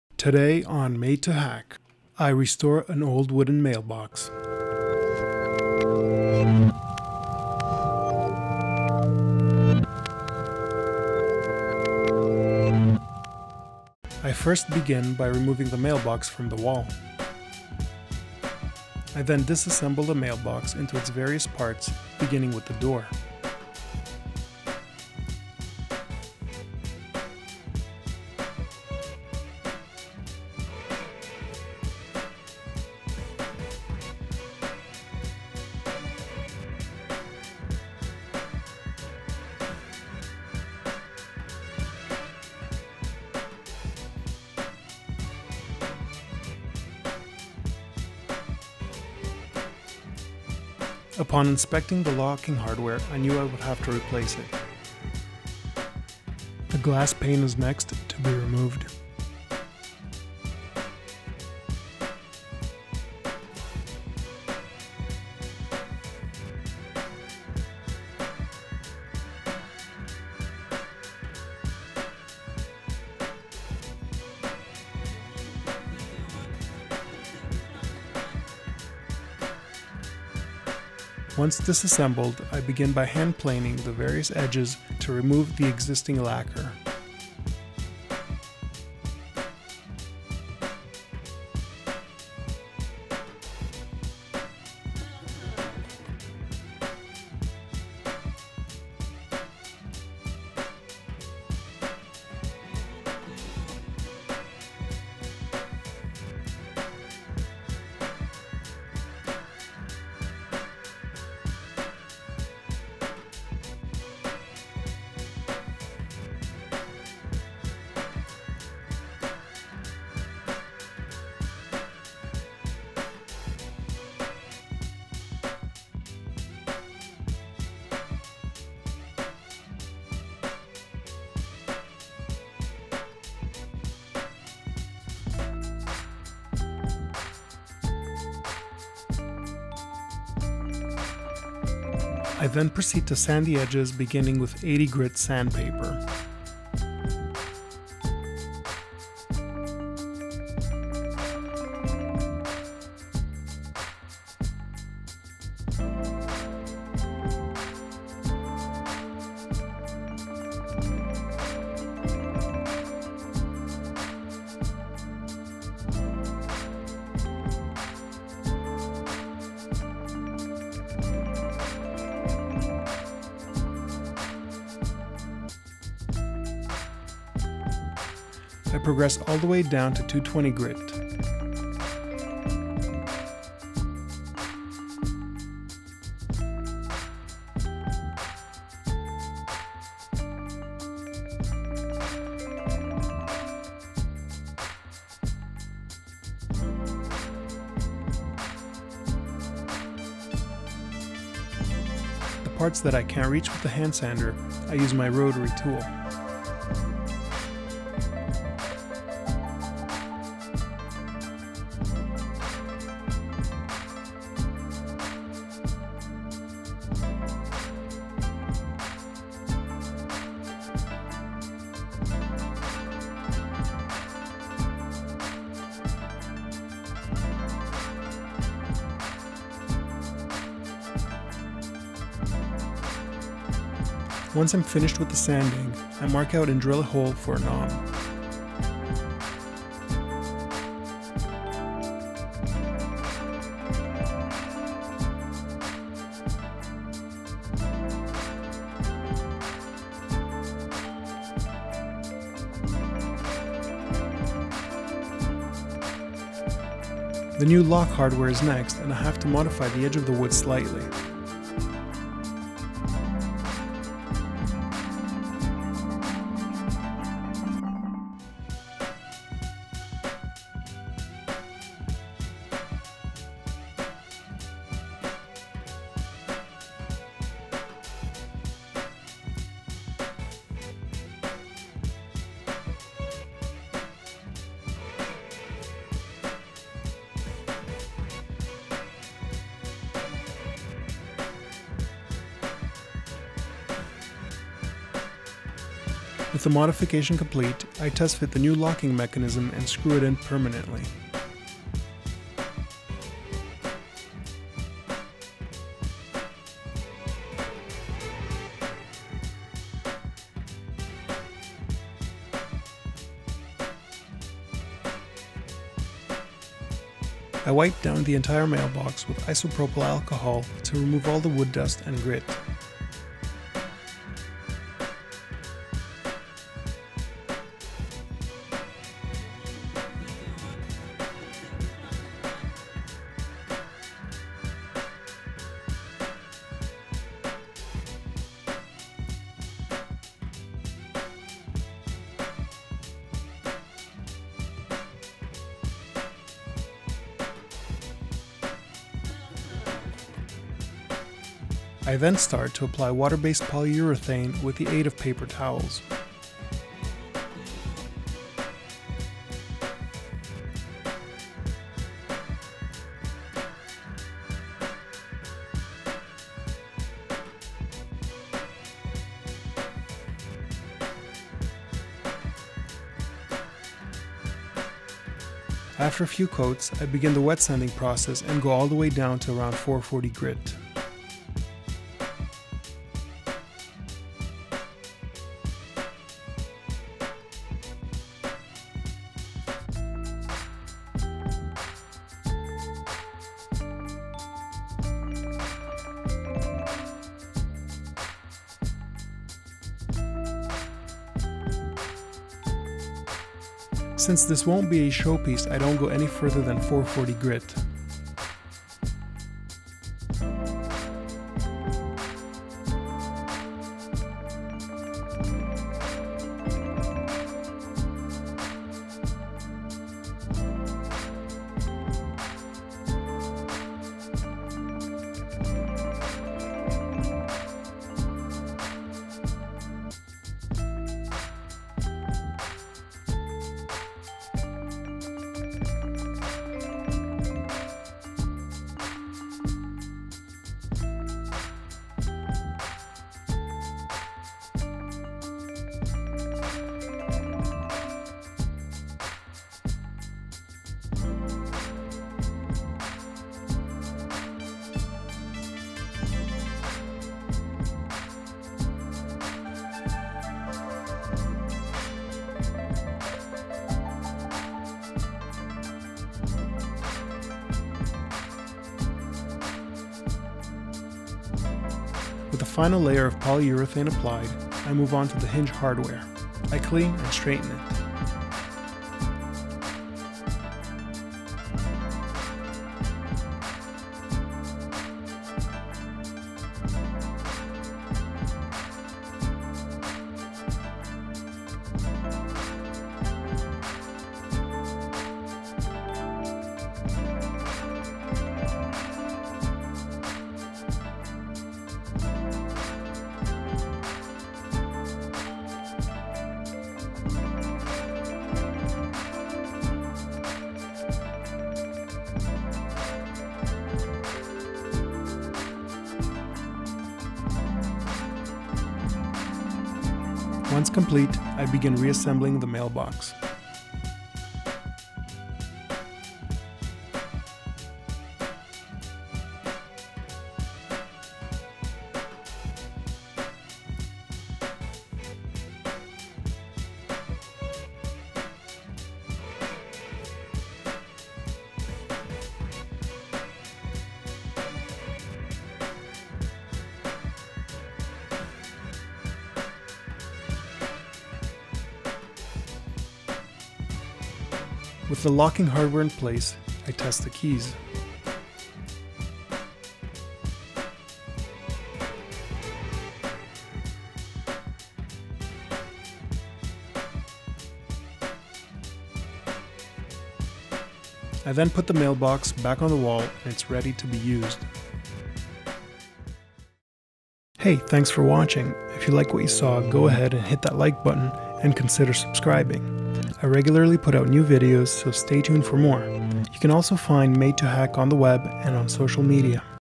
DIY - How To - Wooden Mailbox Restoration
I show you how I restored an old wooden mailbox to its former sheen.